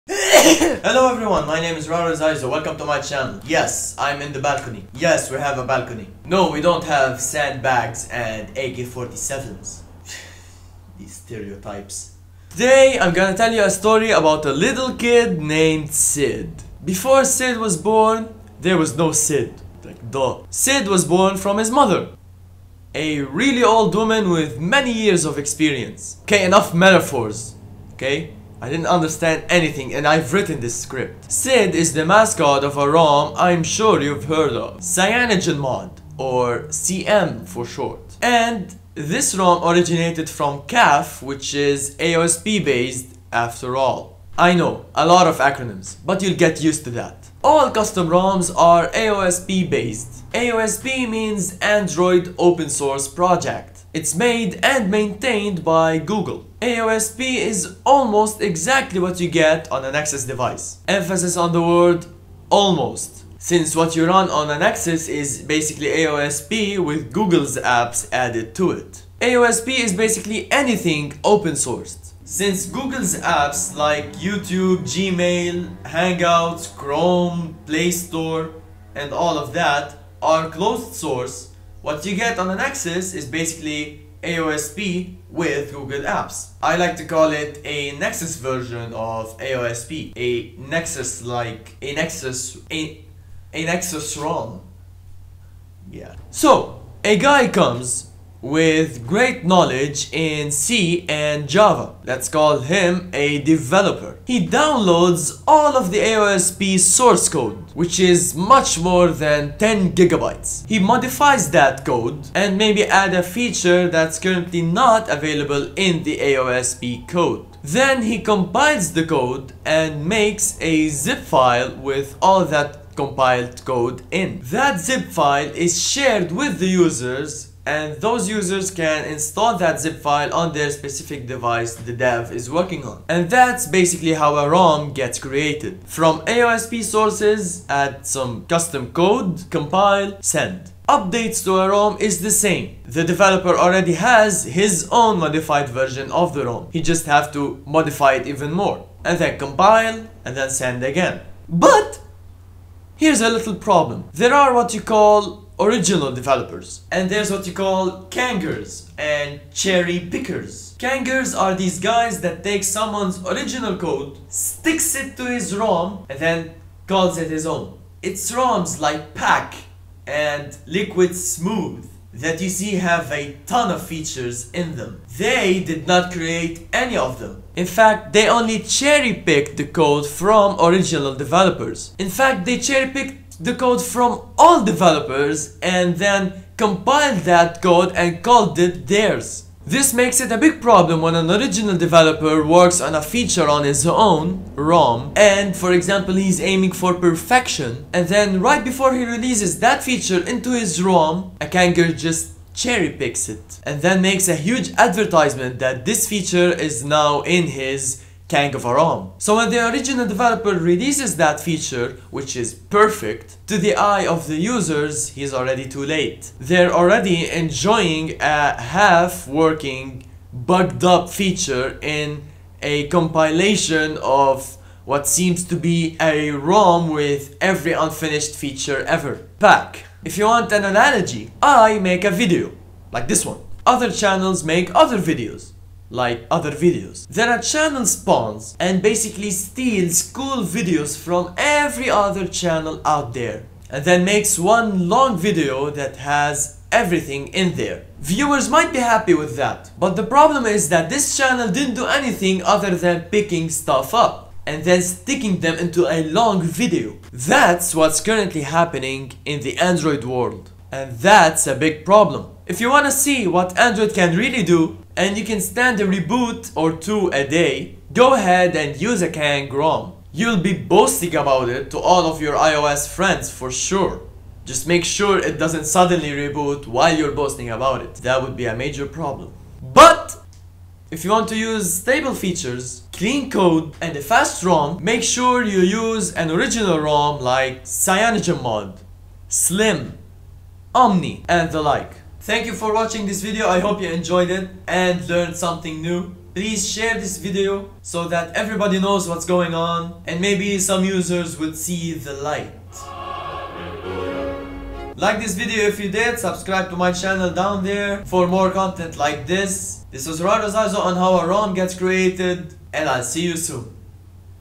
How a ROM Gets Created
How a ROM gets created, without going deep into the source codes and compilation process. And also I talk about a major problem in the android world.